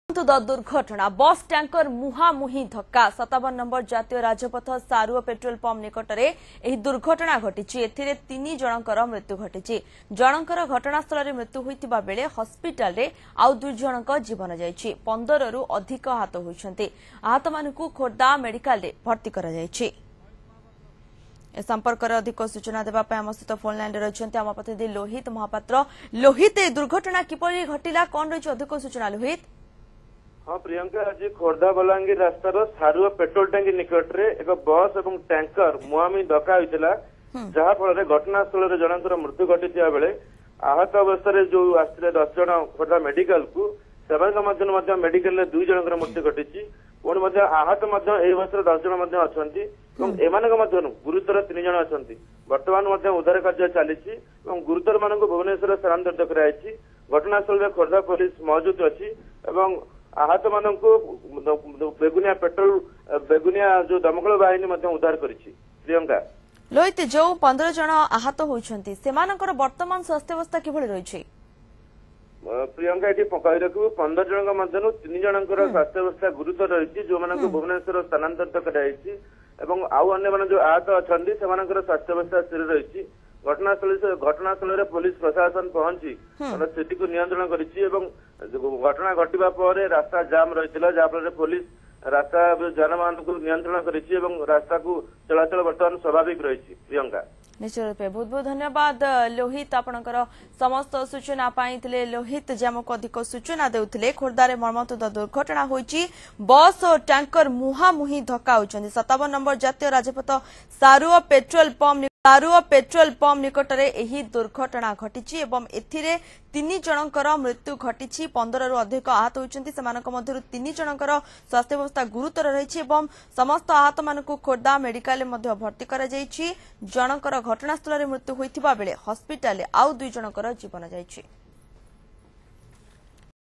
ଖୋର୍ଦ୍ଧାରେ ବସ୍-ଟ୍ୟାଙ୍କର ମୁହାଁମୁହିଁ ଧକ୍କା, ୧ ମୃତ, ୧୦ରୁ ଅଧିକ ଆହତ
ବସ୍-ଟ୍ୟାଙ୍କର ମୁହାଁମୁହିଁ ଧକ୍କା, ୧ ମୃତ, ୧୦ରୁ ଅଧିକ ଆହତ
ଆହତଙ୍କୁ ଖୋର୍ଦ୍ଧା ମେଡିକାଲରେ ଭର୍ତ୍ତି କରାଯାଇଛି
୫୭ ଜାତୀୟ ରାଜପଥ ସାରୁଅ ପେଟ୍ରୋଲ ପମ୍ପ ନିକଟରେ ଦୁର୍ଘଟଣା
ବସଟି ଭୁବନେଶ୍ବରରୁ ବାଙ୍କି ଯାଉଥିଲା